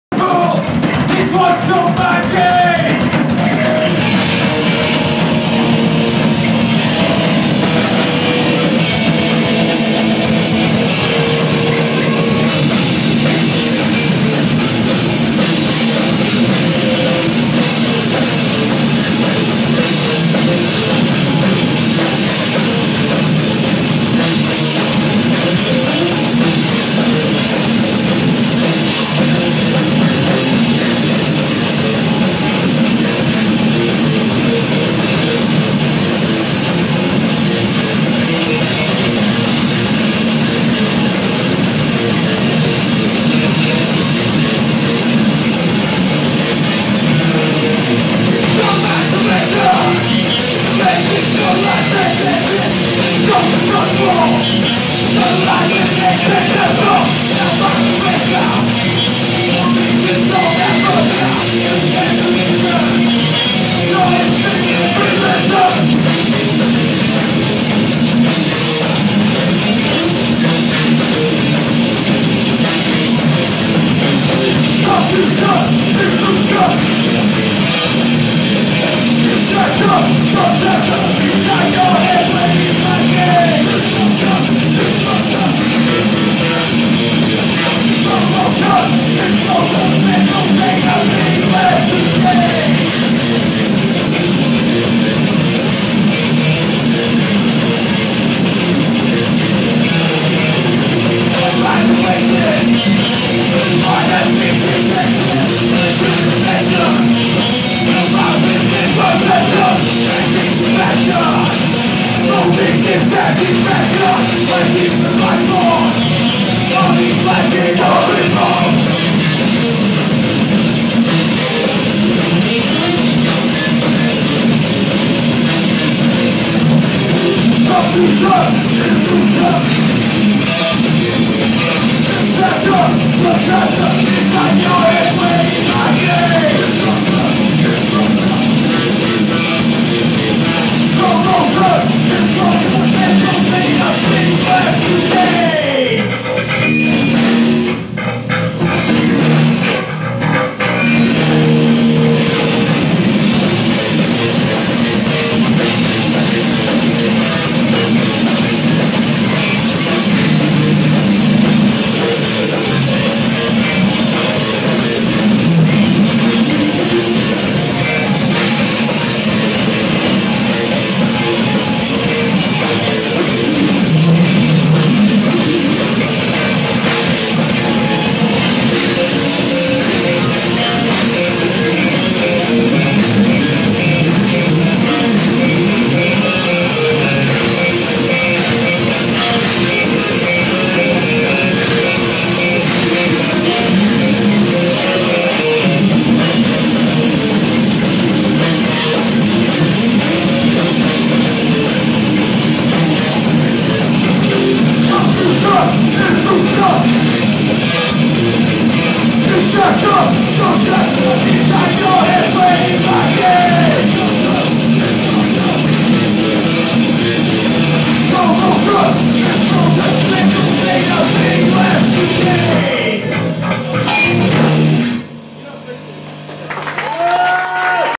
Exiled
Exiled start of set @ Retro Bar, Friday 8 May 2015. Awesome lads